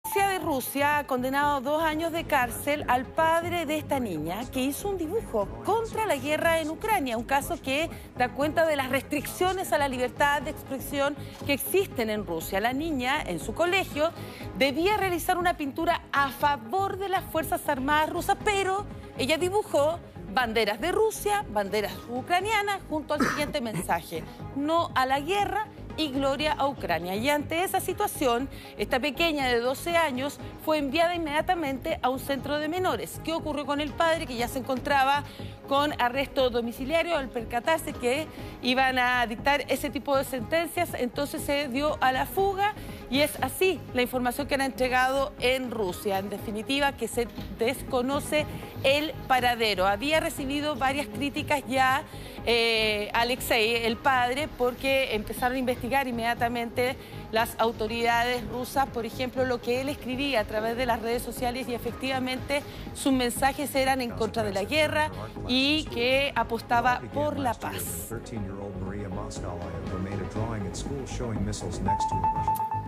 Condenan a padre por dibujo de su hija en Rusia: Era en favor de Ucrania y no a la guerra
Condenan a dos años de cárcel al padre de una niña por un "polémico" dibujo en Rusia. La menor de edad, en un trabajo escolar, tenía que elaborar una ilustración a favor de las fuerzas armadas rusas, pero decidió hacer el trabajo en alusión a la paz y en favor de Ucrania.